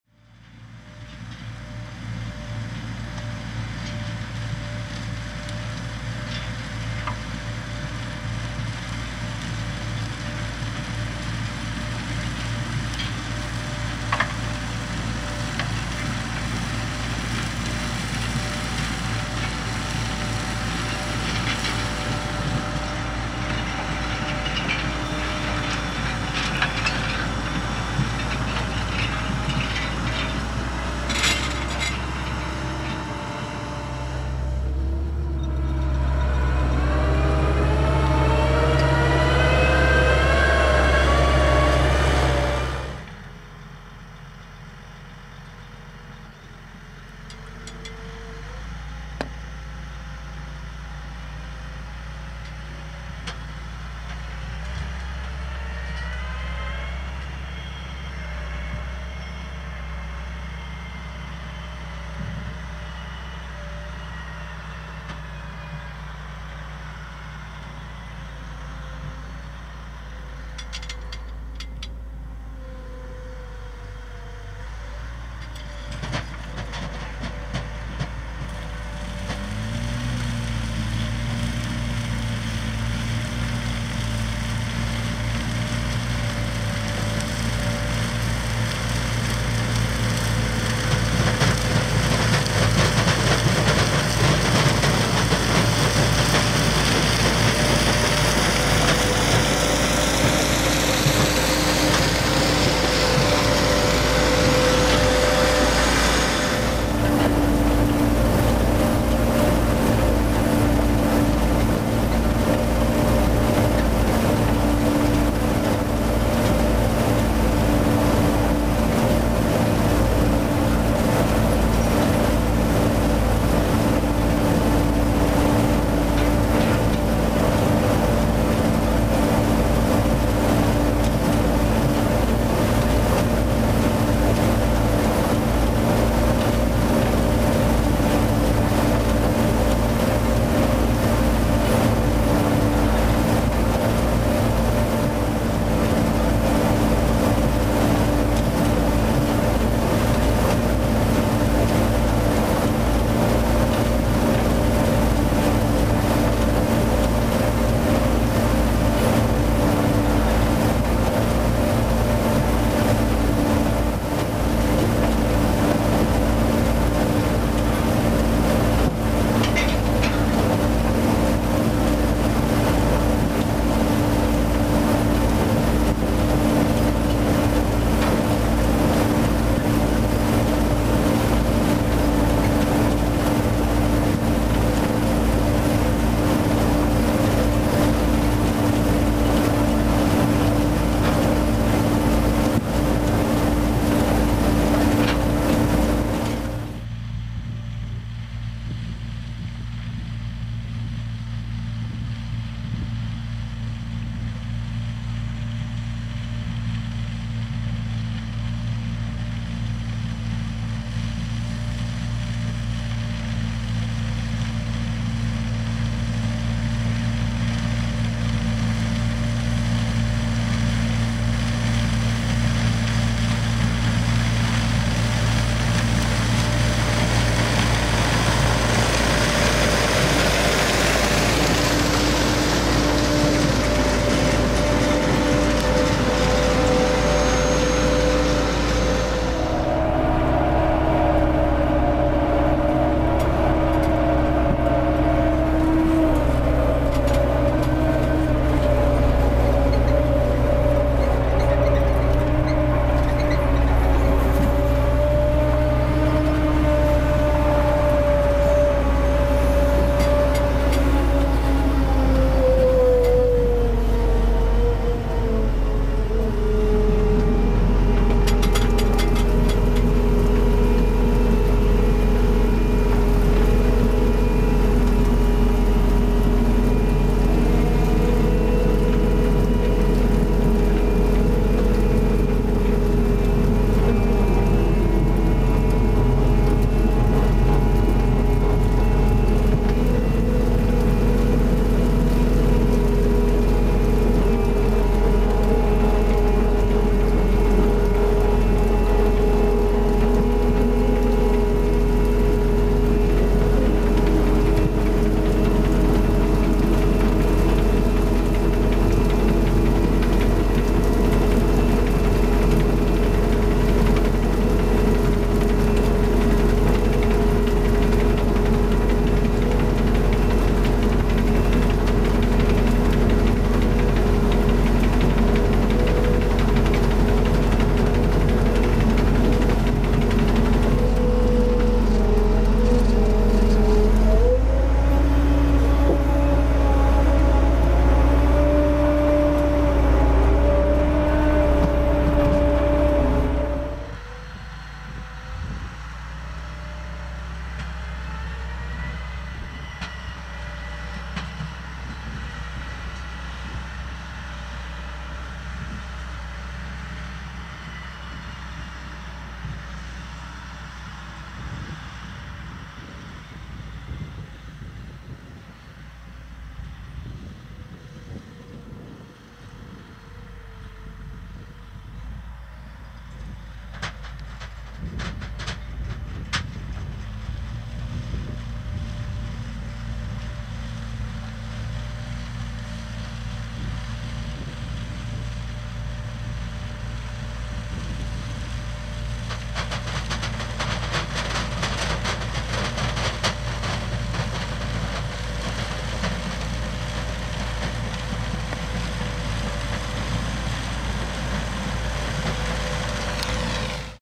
Rübenernte 2022 mit Ropa Tieger 6 | LKS Starkenberg
Zuckerrübenernte mit  einem Ropa Tieger 6 von LKS Starkenberg